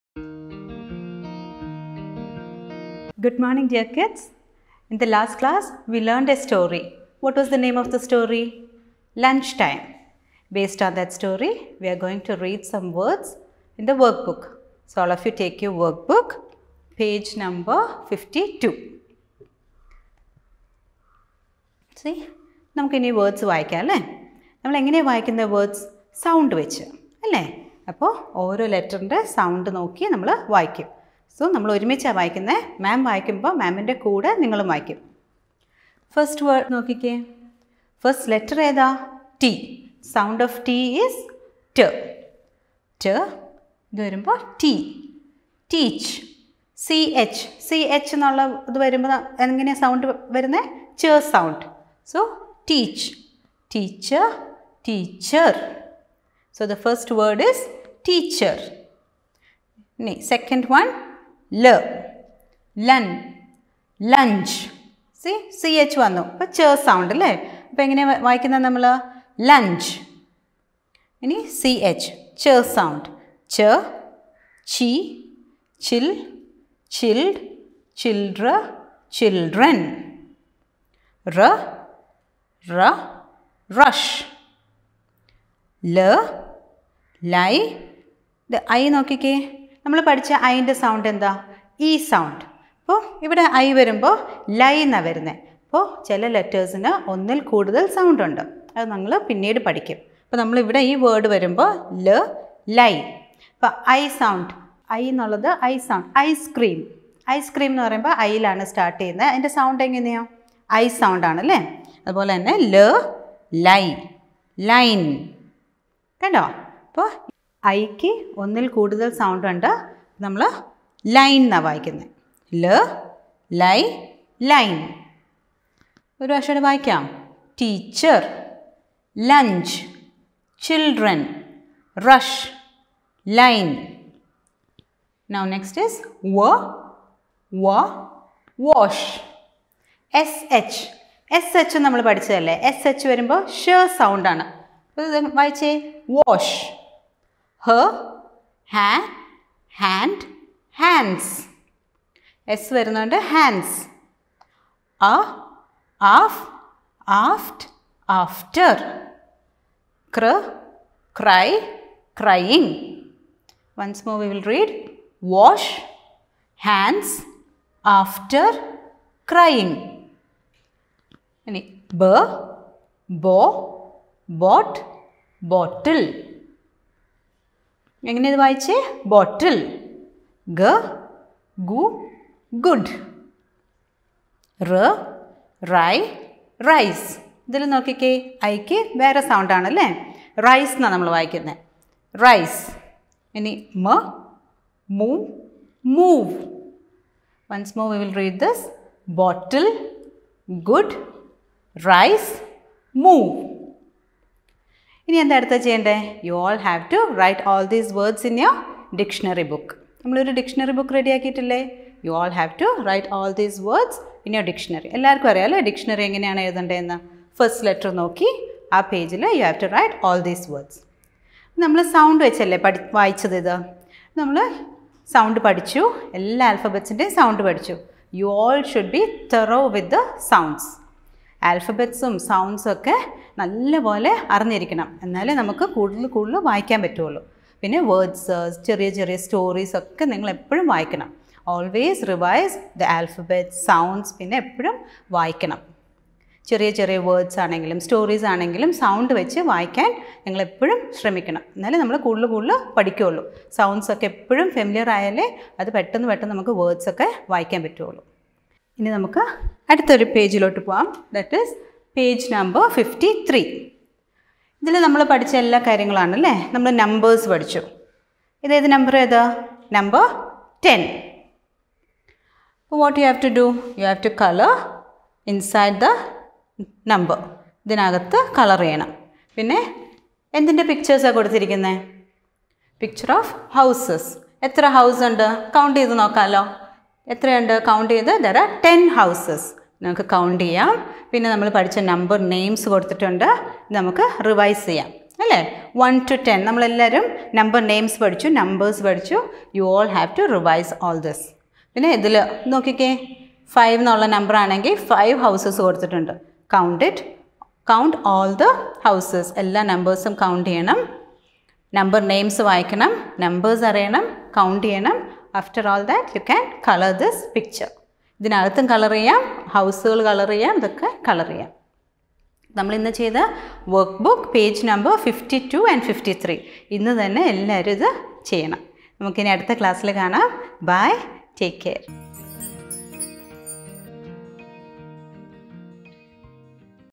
Prep 2 Vocabulary practice on workbook page no 52, 53
Online classrooms for Prep 2 students of The Garden School. Subject: English.